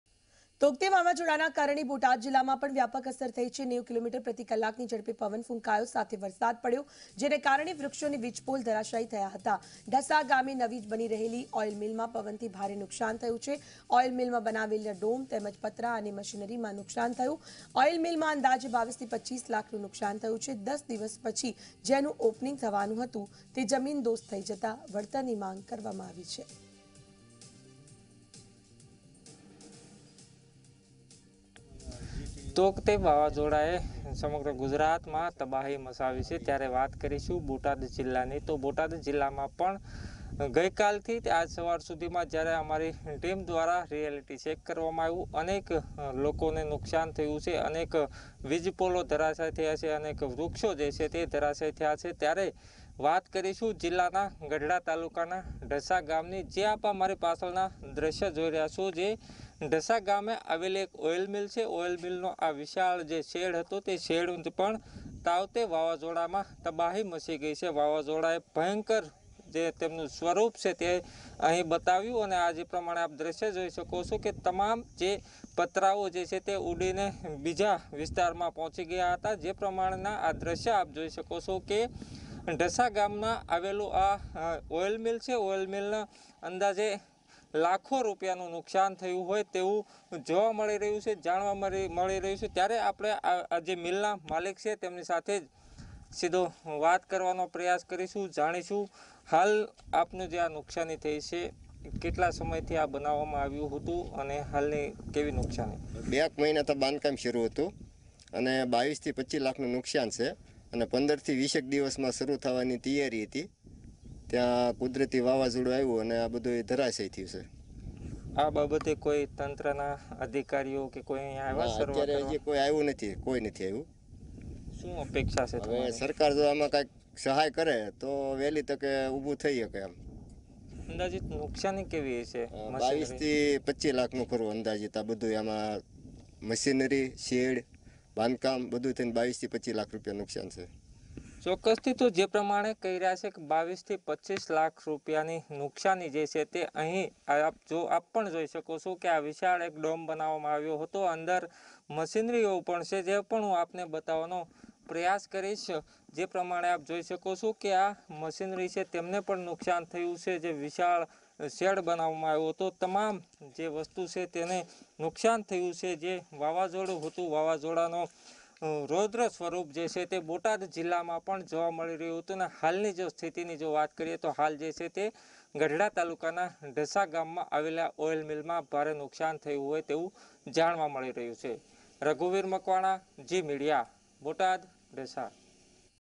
Botad માં Tauktae Cyclone ના કારણે Oil મિલને નુકસાન | Gujarat News | Watch on ZEE 24 Kalak
તૌકતે વાવાઝોડાએ જિલ્લાના તમામ તાલુકાઓ અને ગ્રામ્ય વિસ્તારમાં અસર જોવા મળી હતી. ૯૦ કિમી ની ઝડપે પવન સાથે ધોધમાર વરસાદ પણ પડ્યો હતો જેના કારણે વૃક્ષો, વીજપોલ સહિતનુ નુકસાન થયું છે ત્યારે ગઢડા તાલુકાના ઢસા ગામે નવી બની રહેલ ઓઈલમીલ મા પતરાનો ડ્રોમ તેમજ મશીનરી મા અંદાજે ૨૨ થી ૨૫ લાખનુ નુકસાન થયું છે ત્યારે સરકાર દ્વારા વાવાઝોડા મા નુકસાન થયેલા લોકોને વળતર આપવા લોકોએ માંગ કરી છે.